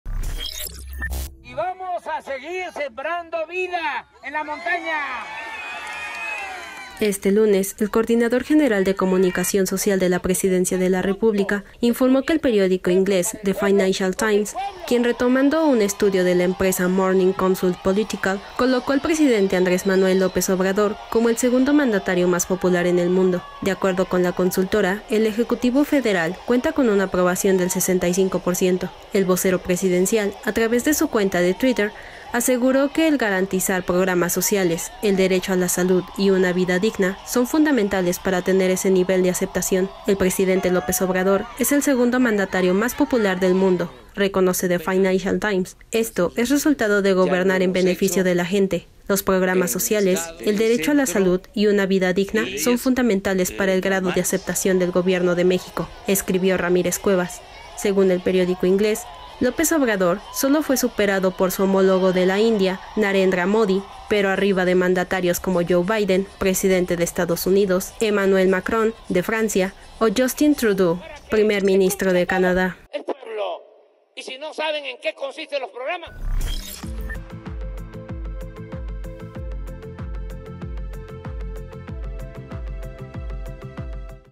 AMLO es el segundo mandatario más popular del mundo: Presidencia
López Obrador sólo fue superado por su homólogo de la India, Narendra Modi, asevera el vocero de Presidencia, quien publicó un artículo del periódico inglés The Financial Times